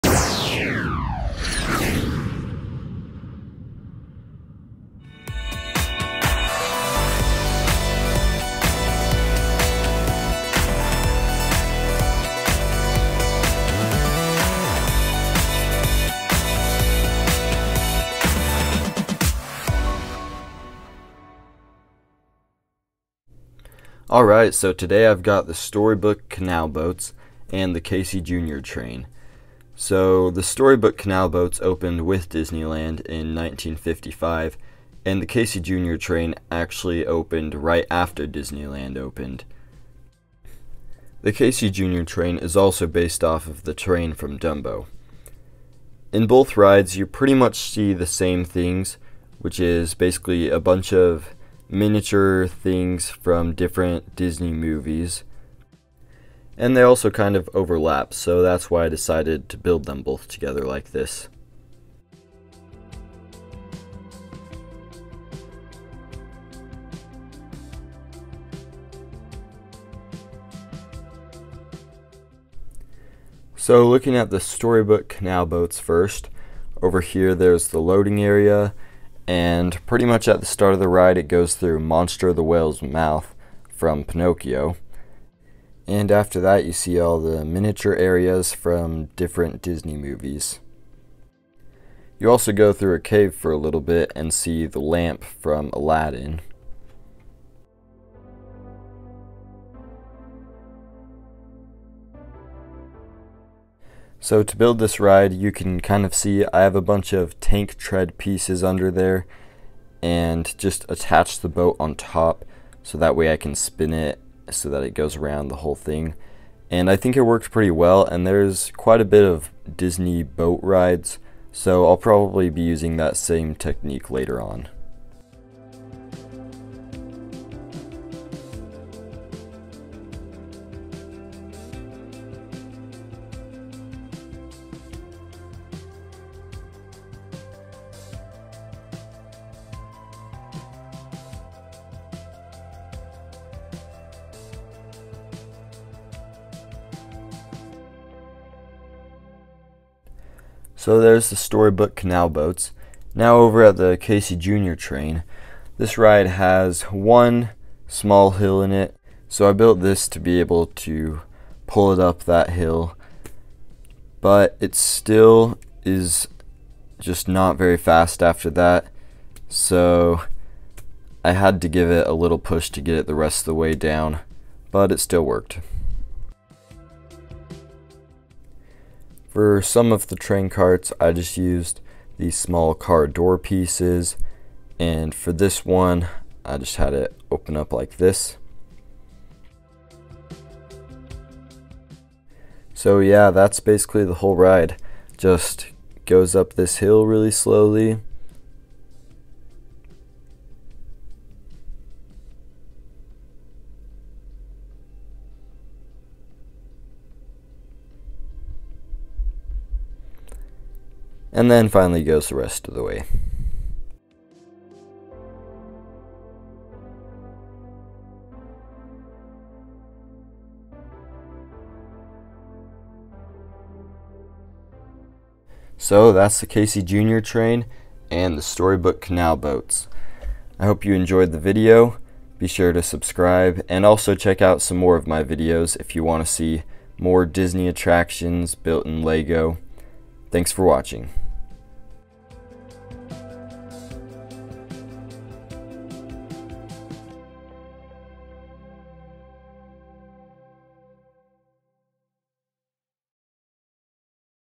LEGO Casey Jr Train and Story Book Disney Rides
Disneyland Story Book Canal Boats and Casey Jr Train rides, boat ride and short train ride built in LEGO
MOC